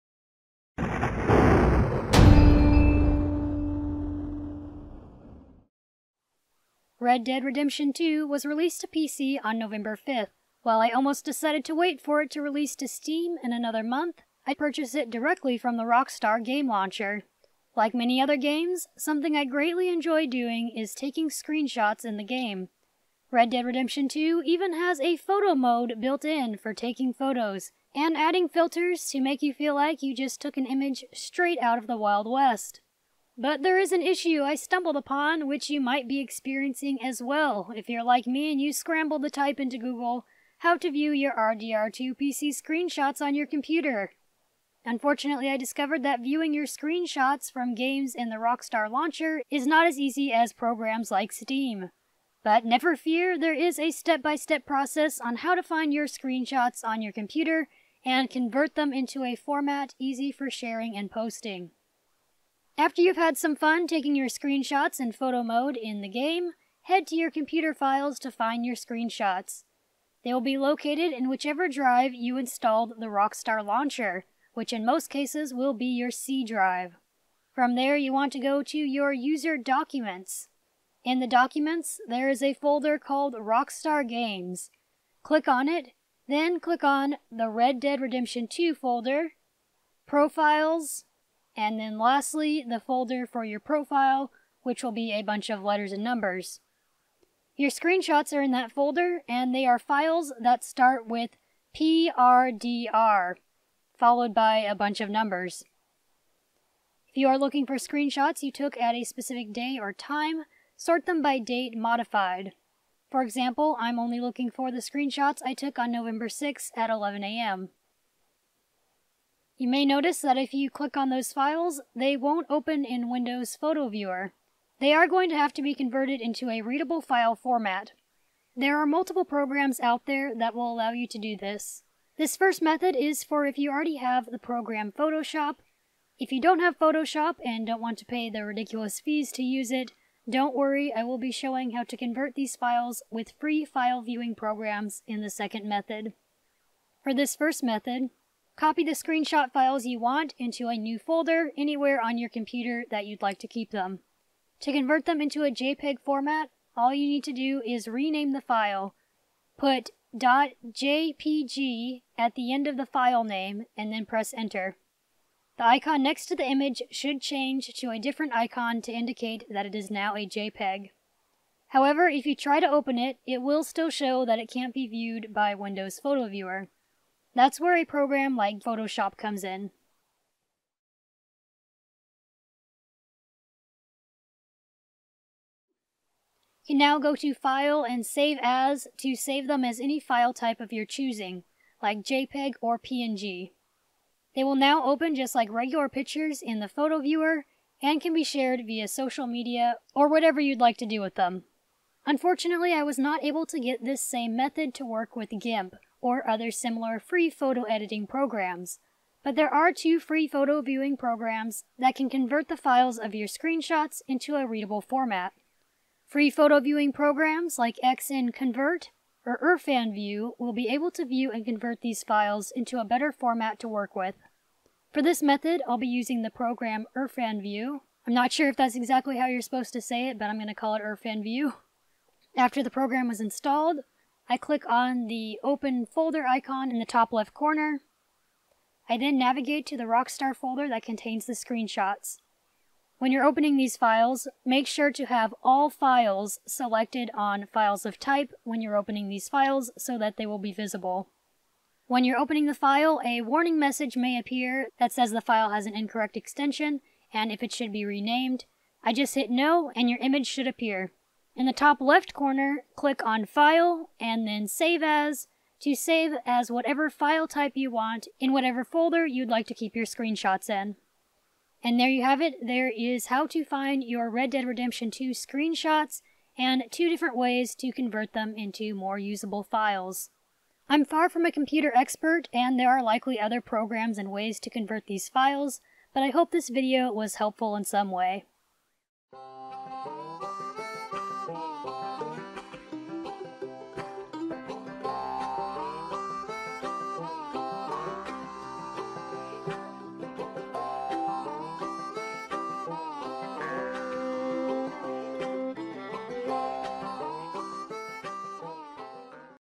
How to Find Your Red Dead Redemption 2 Screenshots on PC (Rockstar Launcher)
Tutorial on how to locate, view, and convert your RDR2 screenshots into JPEG or PNG format on Windows computers if you're playing the game from the Rockstar Game Launcher.

Tutorial done on a Windows 10 PC.
Photoshop version 7.0 used in video (ancient version, I know.)

~~ LINKS ~~